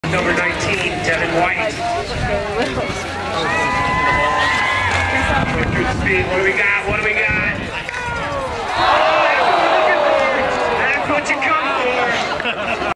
Devon White Spring Splash 2013
Outstanding Crashing and Splashing at Colorado's Spring Splash at Winter Park, CO.  Super Funny Crash, Spills, and Thrills - Costumes, Hilarious Face Plants and More Comedy..!